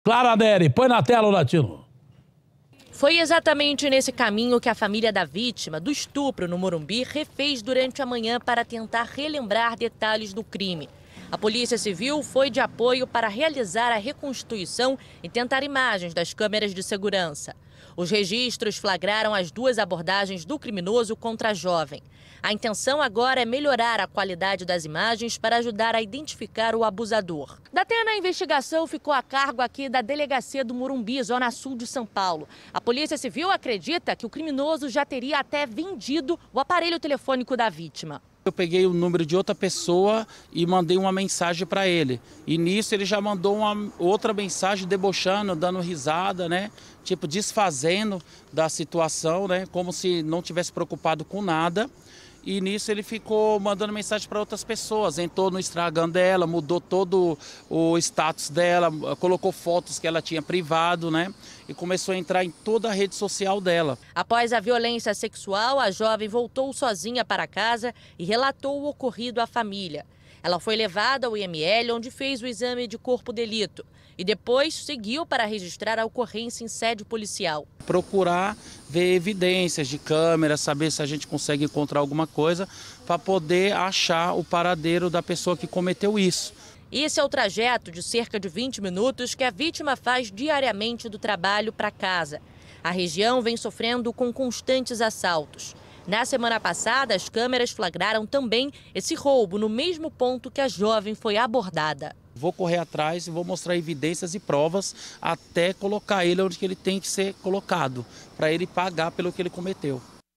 SEQUESTRO E ABUSO NO MORUMBI
Família da jovem atacada tem recebido mensagens de deboche após crime.